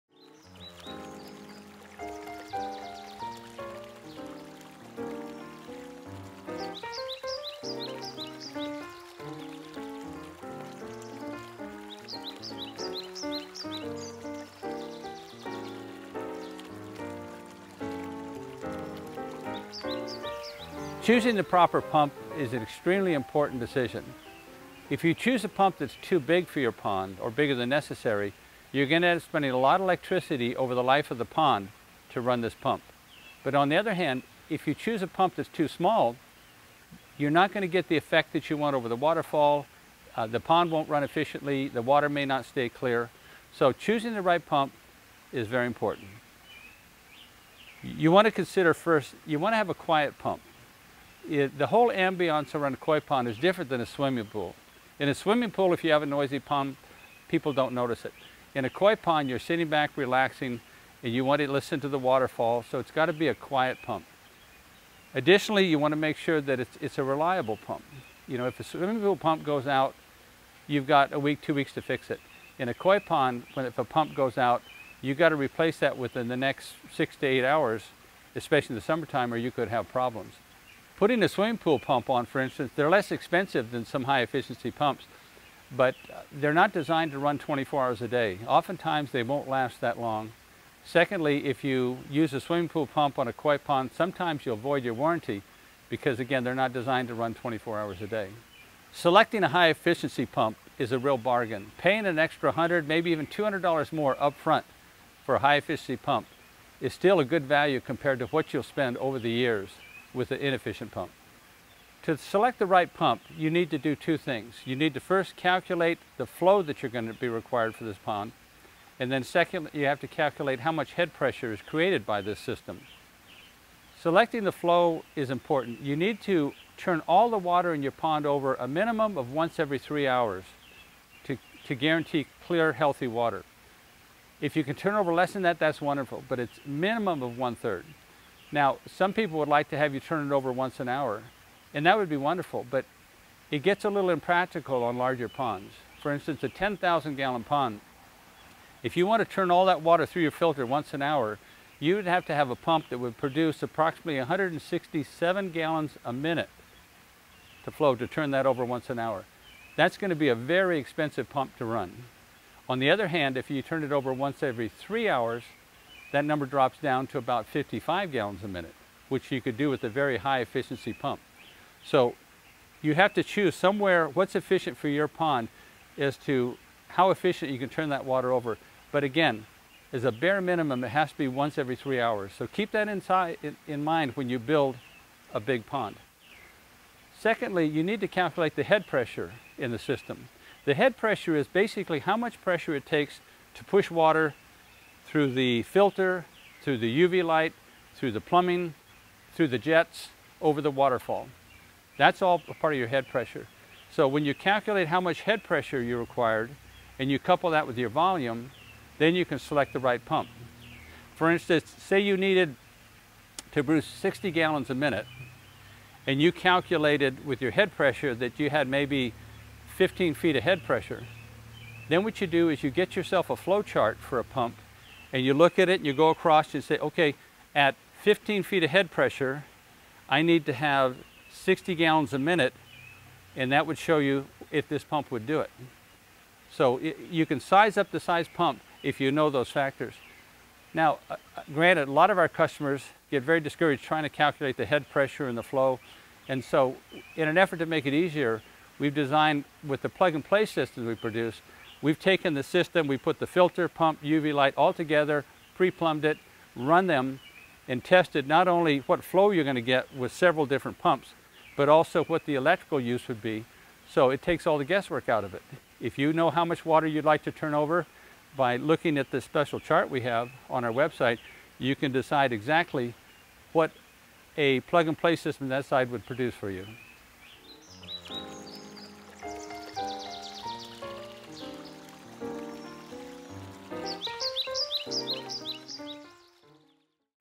What size pump will I need?
A correcltly sized pump is extremly important for any level of pond enthusiat. Choose one too small and It will now work as intended. Choose one to large and you wasting a significant amount of money each month. This adds up to a great cost over the course of time that you have your pond. Steve Walker, President of Sacramento Koi discusses how to select the correct what sized pump for your ponding needs